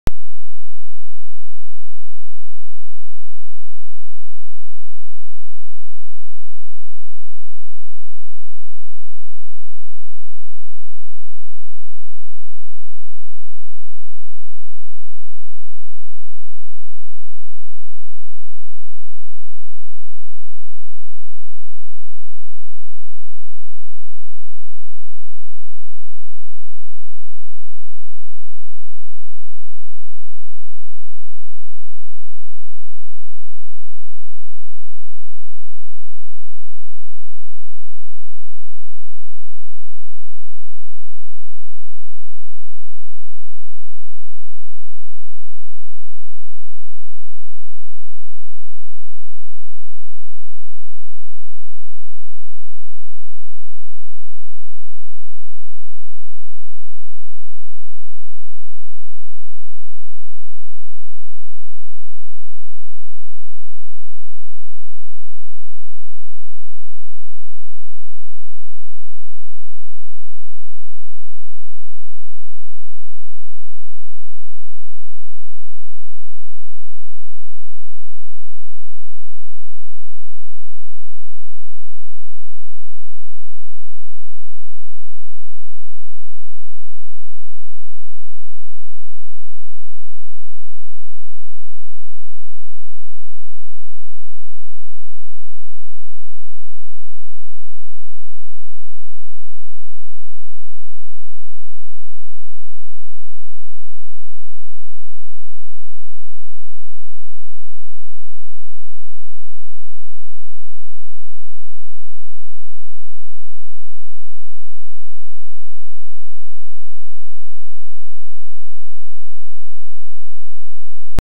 Dumb Ass drivers 3
P plate driver speeding (again)